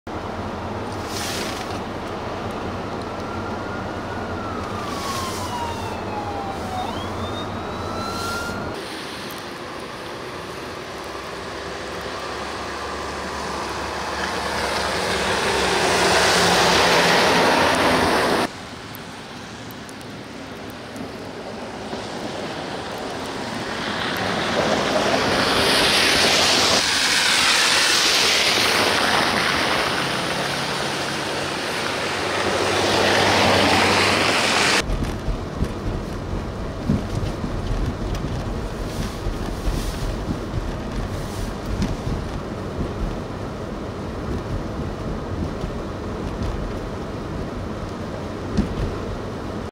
Winter storm begins impacting southern Wisconsin - February 16, 2023
A winter storm begins to impact southern Wisconsin as upwards of 4-7 inches of snow are possible, with locally higher totals possible across the Milwaukee area as lake enhancement could bring snow totals of up to 10" being possible. The following video was captured by @charliebourdo from the Delafield, Wisconsin, area as the winter storm's impacts began.

Youtube #2: @CharlieBourdo2